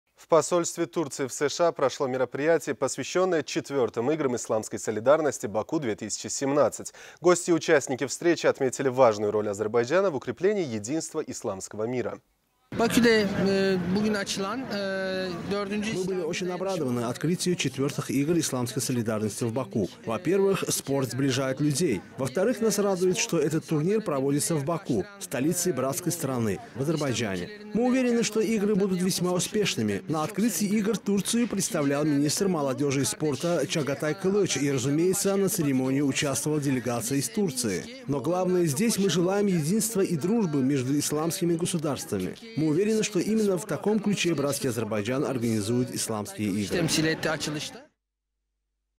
В посольстве Турции в США прошло мероприятие, посвященное IV Играм исламской солидарности в Баку
В посольстве Турции в США прошло мероприятие, посвященное IV Играм исламской солидарности "Баку-2017". Гости и участники встречи отметили важную роль Азербайджана в укреплении единства исламского мира.